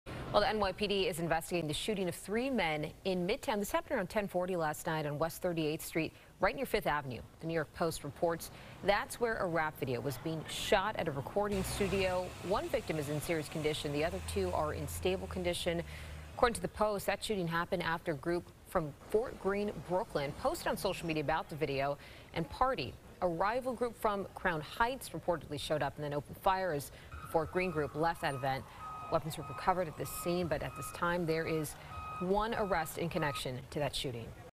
3 men shot near Fifth Avenue
Three men were shot in Midtown Manhattan where a rap video was reportedly being filmed. One person was arrested.  Two of the victims were in stable condition and one in serious condition at Bellevue Hospital.